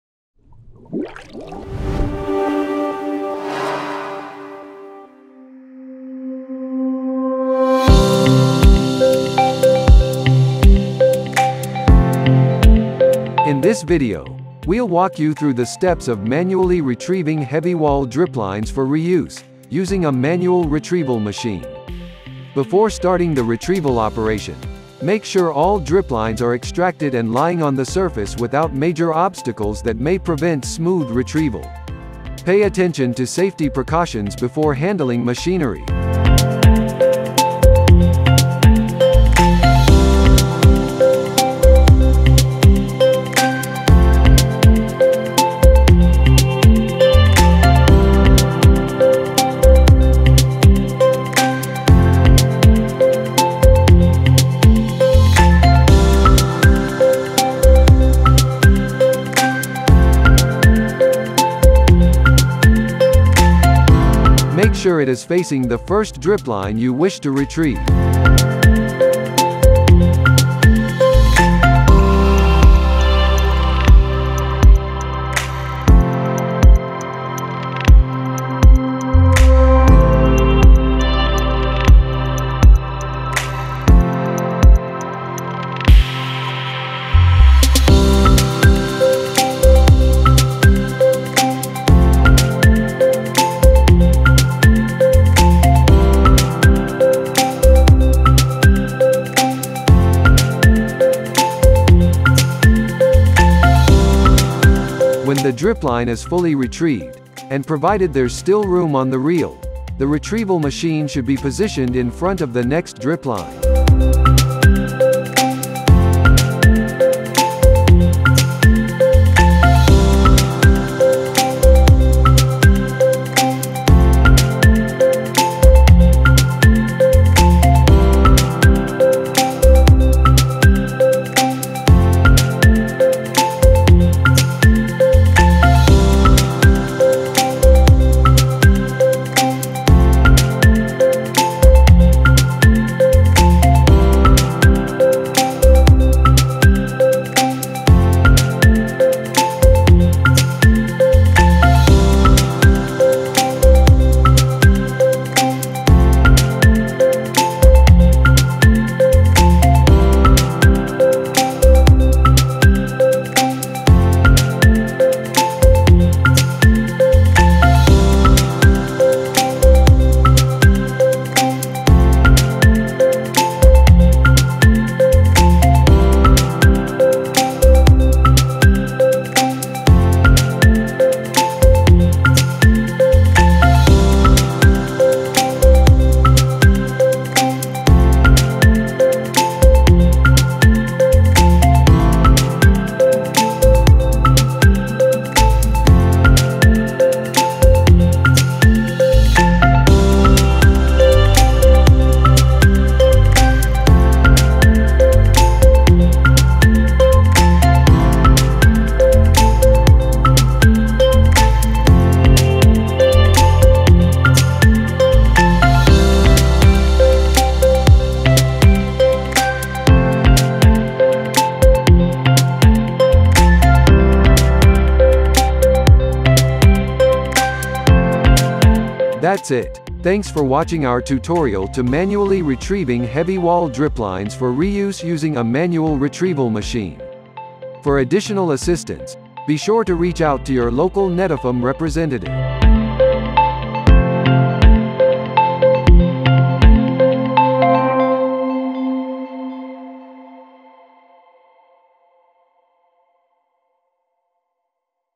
How to Retrieve Heavy Wall Dripline (HWD) for Reuse Using a Manual Retrieval Machine
Follow this tutorial to learn how to manually retrieve Heavy Wall Driplines (HWD) for reuse using a Netafim manual retrieval machine and We'll guide you through the entire stages.
For additional assistance, contact our local Netafim representative and check our